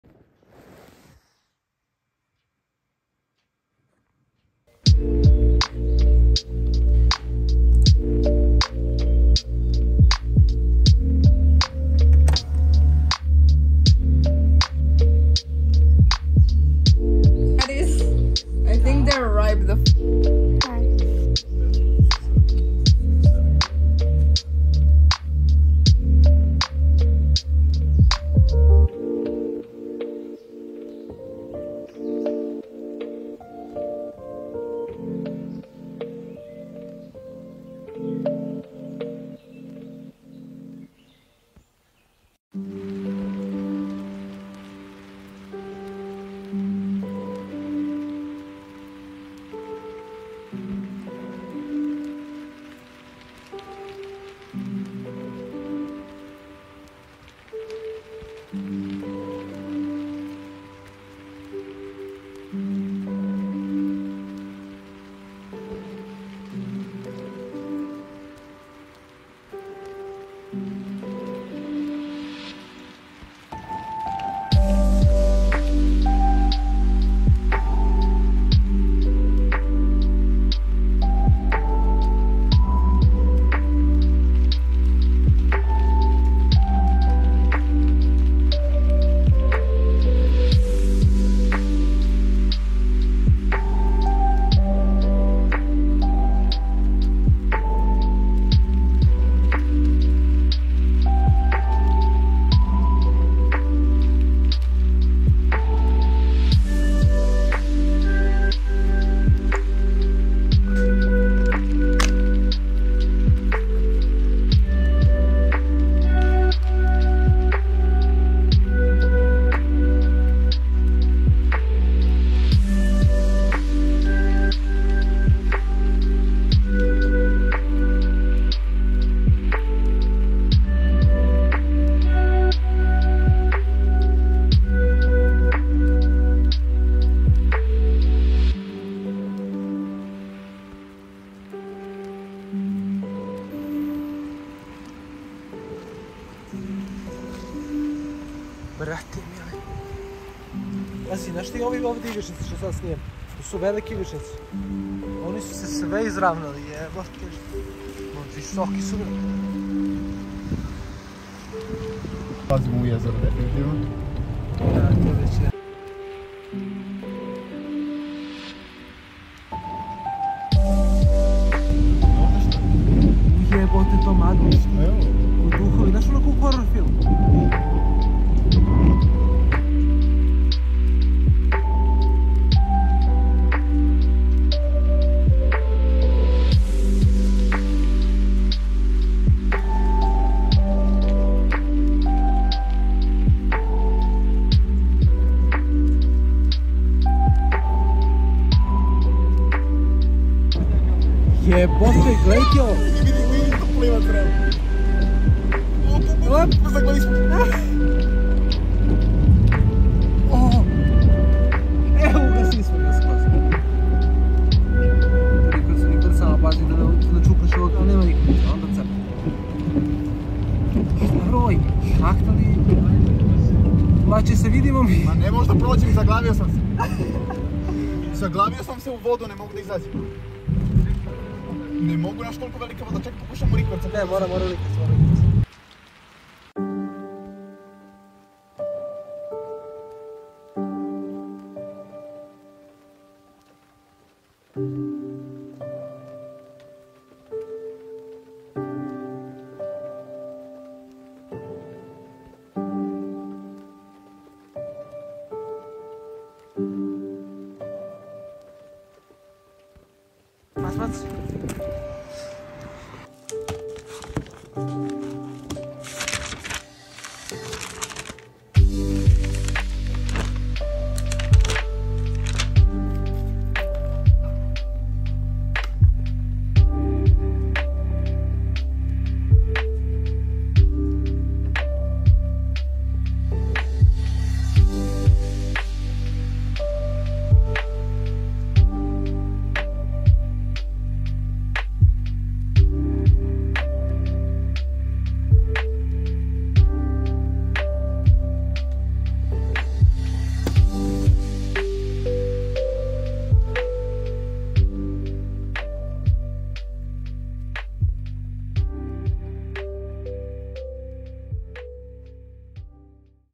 University Study Vlog: A Day in the Life of a Student Software Engineer In Serbia
This whole video is shot using iPhone 13 Pro

My Gear:
Canon 4000D 50mm 1.8
Iphone 13 Pro
Macbook Air M1 Late 2020